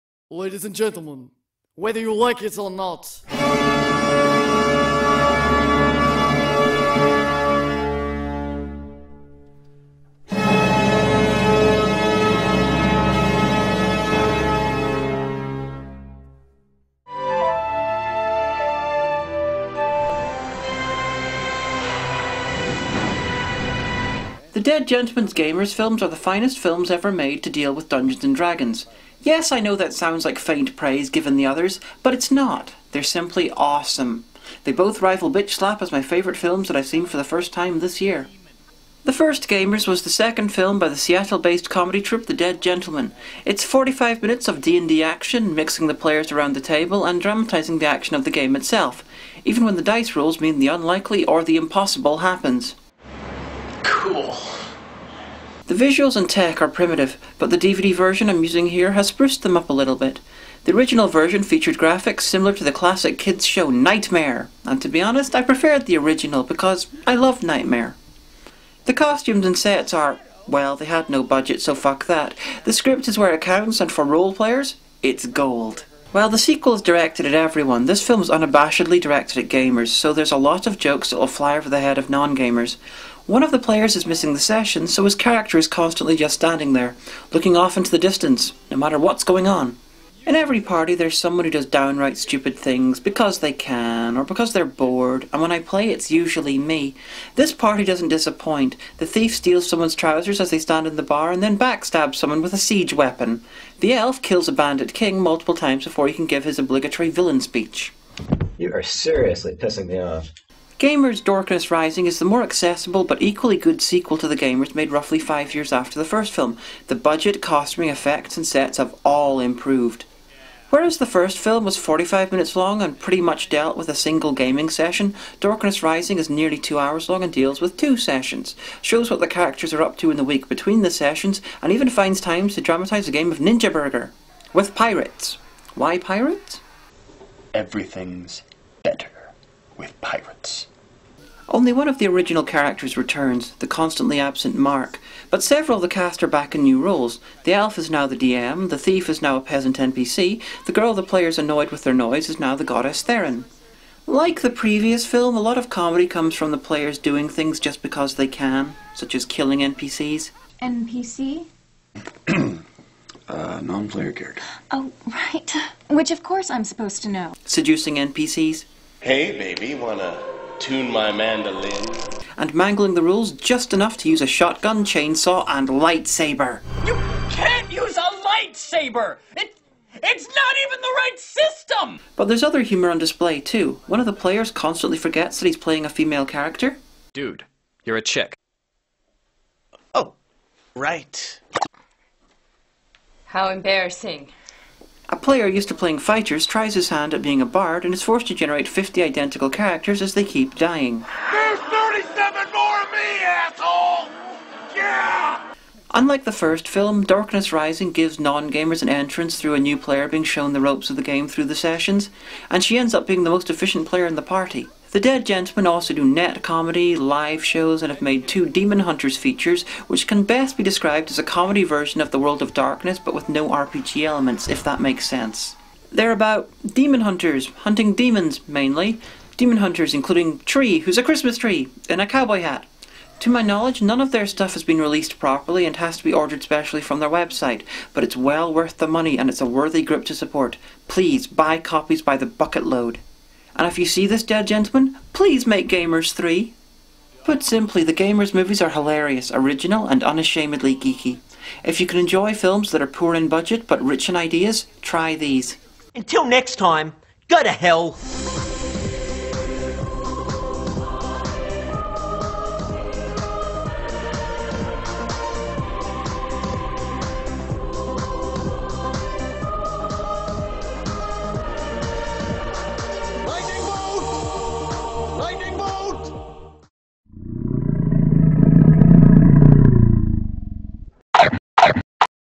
Plugs for The Gamers and Dorkness Rising
Hagan Plugs the 2 Greatest DnD based Movies ever made! (not that theres that much contest...) The Gamers and The Gamers Dorkness Rising are owned by The Dead Gentlemen.